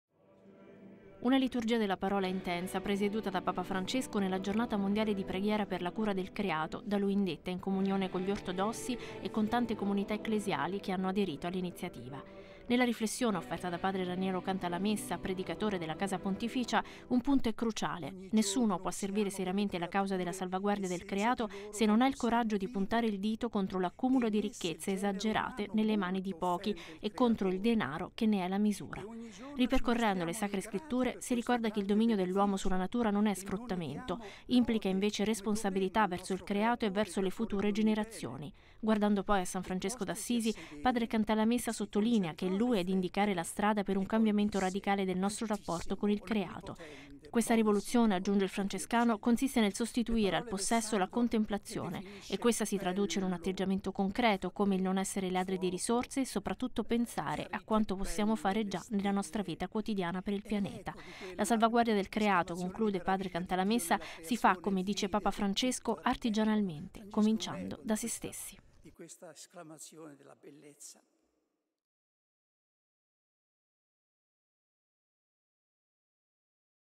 Papa Liturgia Giornata del Creato 01-09-2015
Padre Cantalamessa: si serve il creato se non si accumulano ricchezze esagerate

Nella Basilica di San Pietro, Papa Francesco ha presieduto la celebrazione della prima Giornata mondiale di preghiera per la cura del Creato in comunione con gli ortodossi.